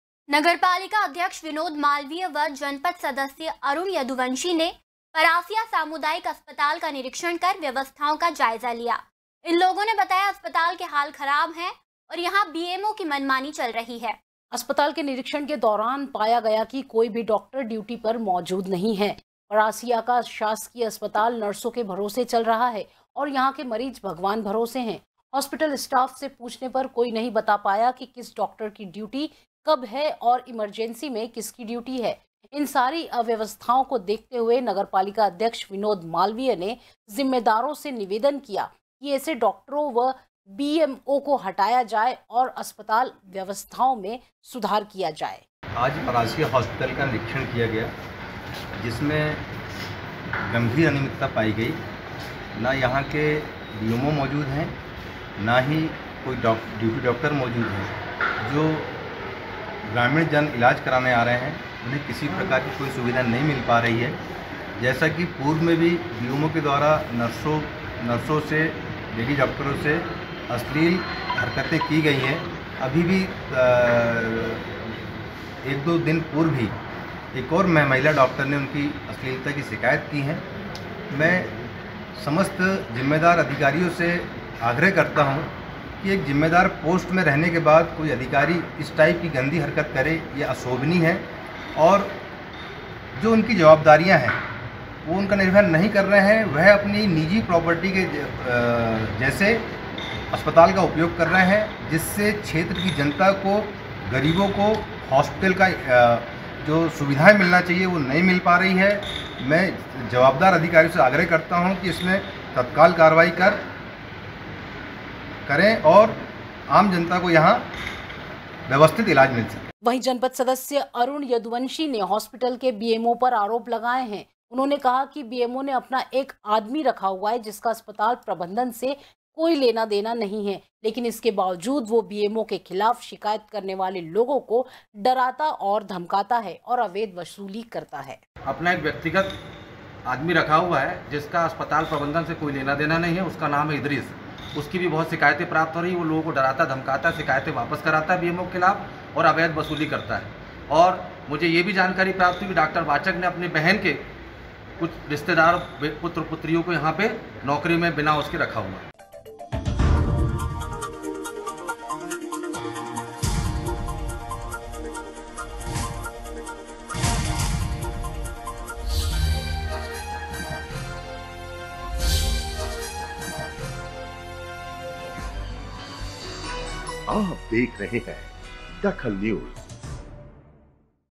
DAKHAL NEWS नगर पालिका अध्यक्ष ने किया अस्पताल का निरीक्षण
नगर पालिका अध्यक्ष ने किया अस्पताल का निरीक्षण
नर्सों के भरोसे चल रहा परासिया सामुदायिक अस्पताल